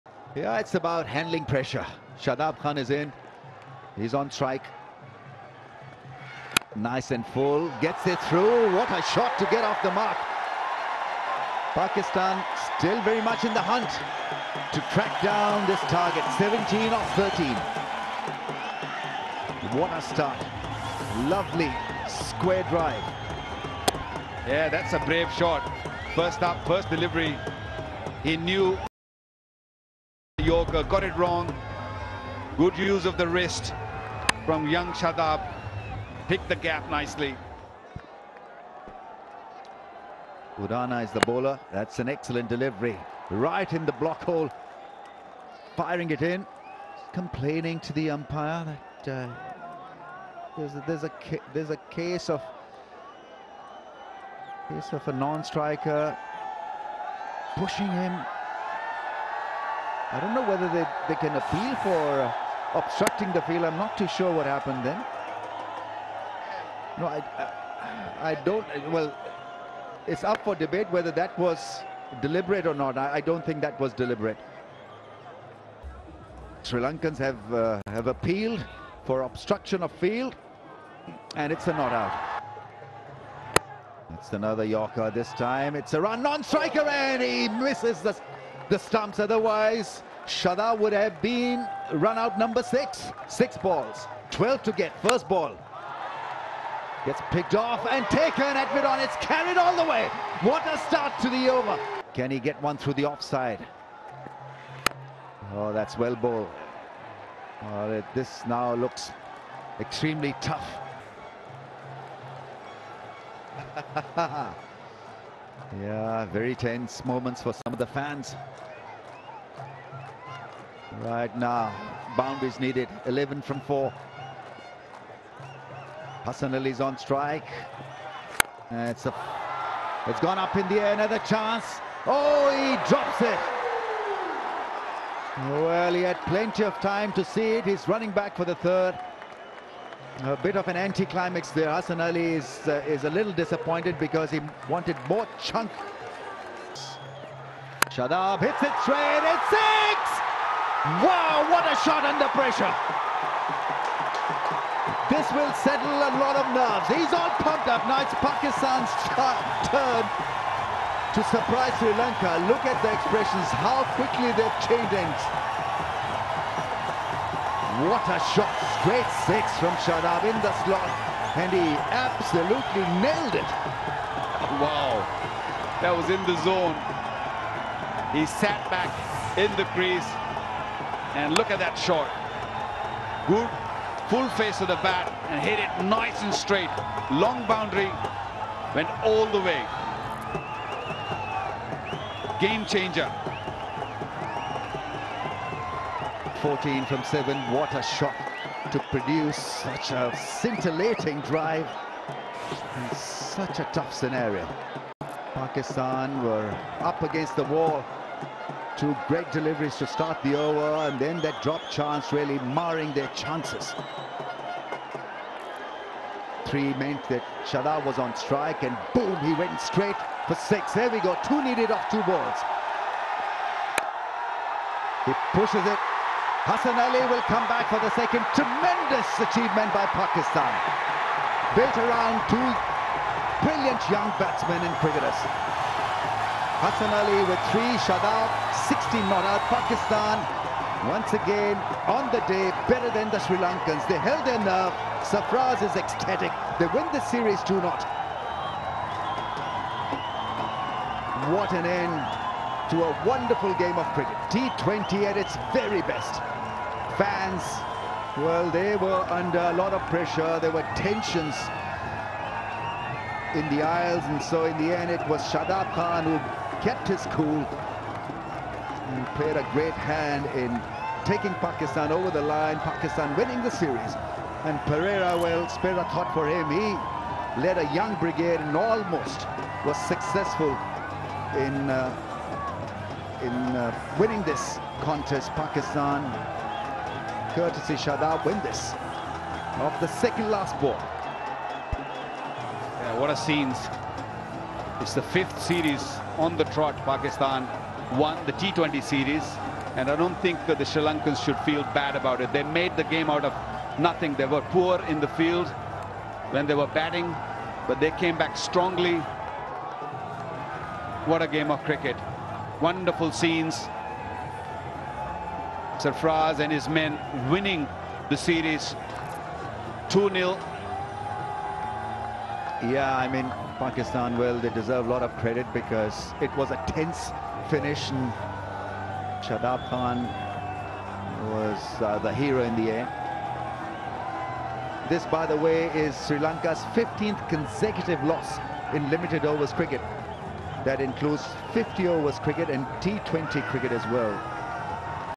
1️⃣2️⃣ Runs Required in 6️⃣ Balls | Shadab Khan Brilliant Fight in Last Over | PCB | MA2A
1️⃣2️⃣ Runs Required in 6️⃣ Balls | Shadab Khan Brilliant Fight in Last Over | PCB | MA2A